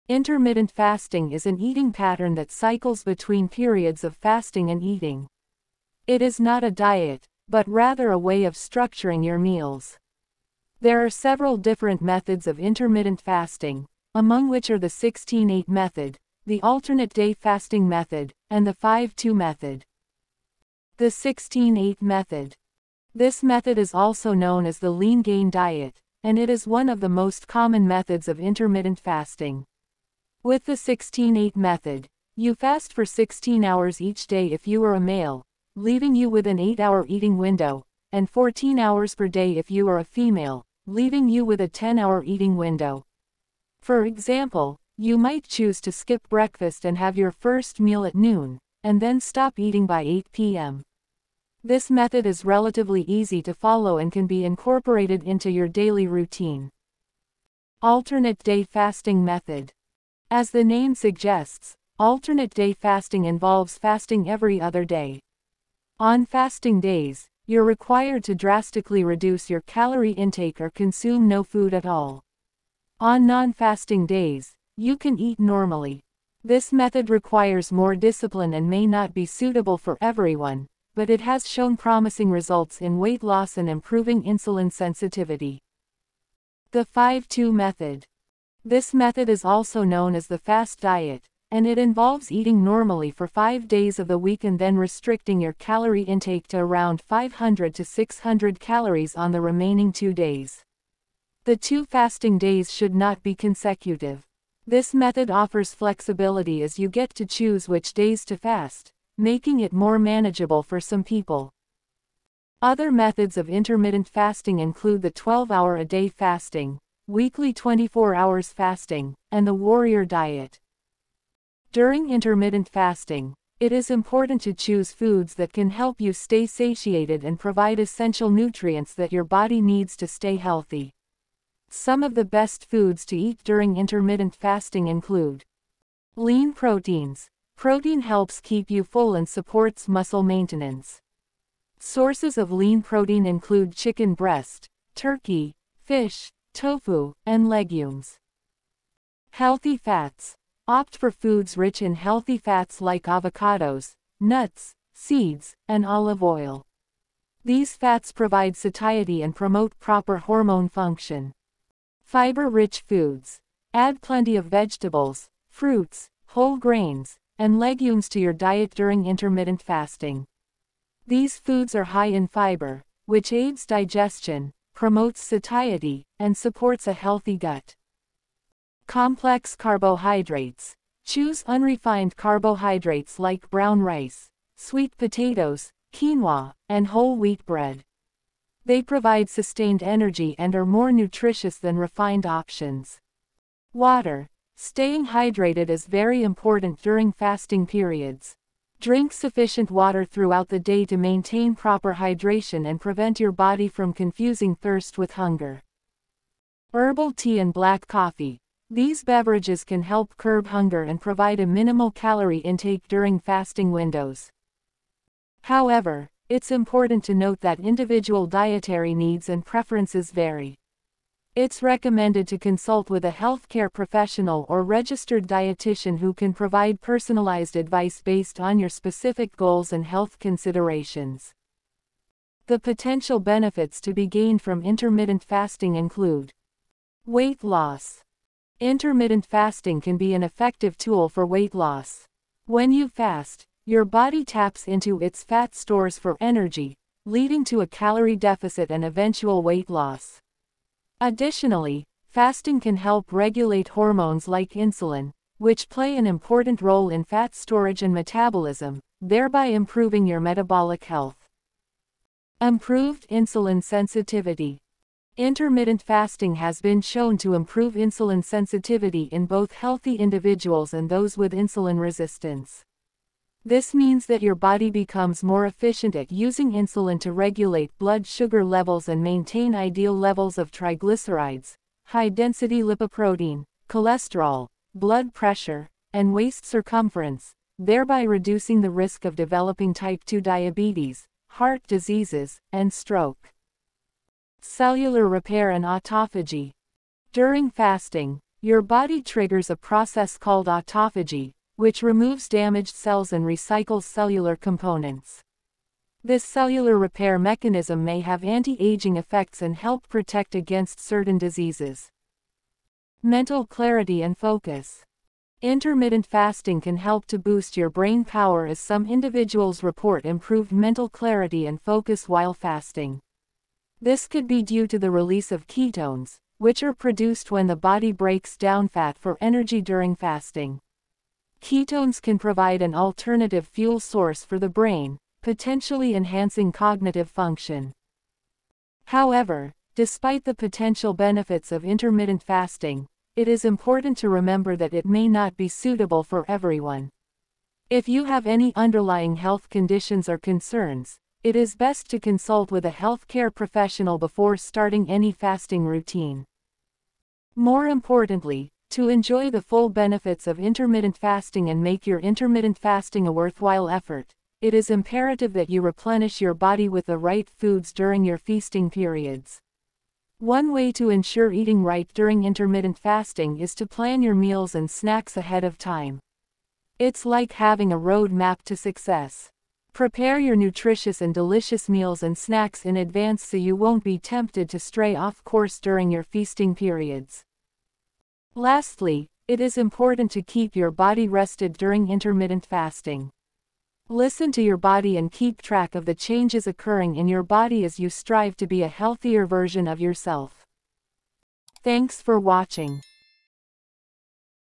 Intermittent Fasting: Methods, The Right Food to Eat, and Benefits
In this video, you will learn about the different methods of intermittent fasting such as the popular 16/8 fasting method, the flexible 5/2 fasting method, and the effective alternate-day fasting method. You will also gain valuable insights into how each method works and choose the one that suits your health condition(s), lifestyle, and goals. This video also provides you with valuable information on the best, nutrition-packed foods to consume during your fasting routine, that will support your health and make your fasting a worthwhile effort. The incredible benefits of intermittent fasting go beyond body weight management, and this video explores how intermittent fasting can help improve your metabolic health, increase your energy levels, and enhance your mental well-being, as well as its potential to prevent chronic health conditions such as heart disease, diabetes, and stroke.

#16/8fastingmethod
#5/2fastingmethod
alternate-dayfasting